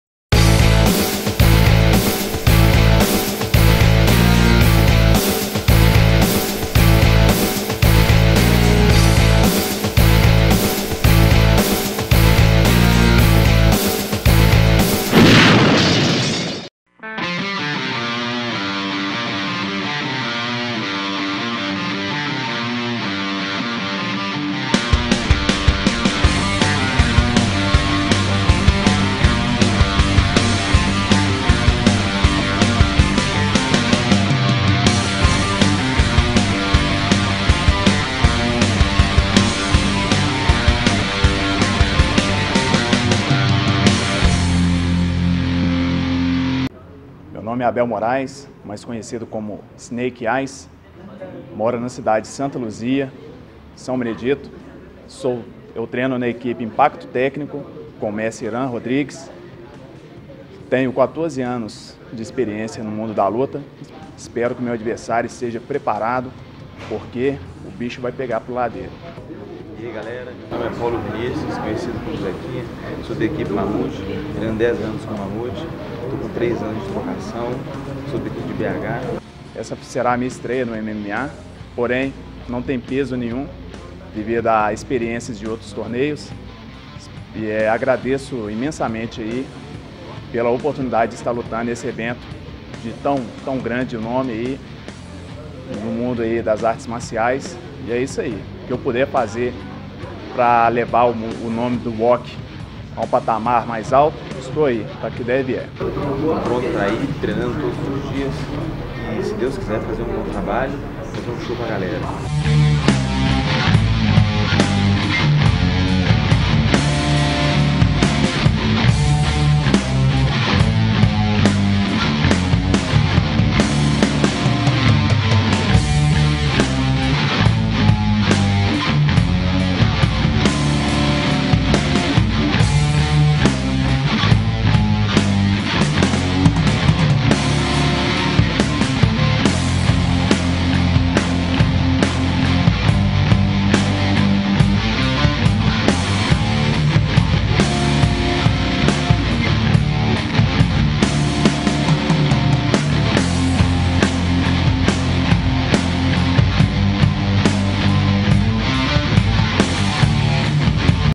Paulo Zequinha encara Snake Eyes na quarta luta do War of Champions 2
Na  quarta luta do War of Champions 2 Abel "Snake Eyes" (Impacto Técnico) encara Paulo "Zequinha" (Mamute Team). Mais experiente dentro do octógono o faixa preta de jiu-jitsu Zequinha conta com vitórias no Mano a Mano, Território Turbo 4x4 e Imperador Fight. Apesar de gostar da luta em pé, o chão pode ser o caminho da vitória para atleta da Mamute Team já que seu adversário vem da trocação. Diferente de Zequinha, Snake Eyes faz sua estreia no MMA dia 15 de novembro e, para ele, a estreia não terá peso por já estar acostumado com os combates no ringue e promete fazer de tudo para conseguir a primeira vitória na carreira e um dia disputar o cinturão do evento.
A segunda edição do WOC será realizada dia 15 de novembro as 20h no Ginásio do Círculo Militar localizado na Av. Raja Gablia, 350, Gutierrez, BH. Os ingressos podem ser adquiridos no valor de R$50,00 (arquibancada) e R$70,00 (área vip).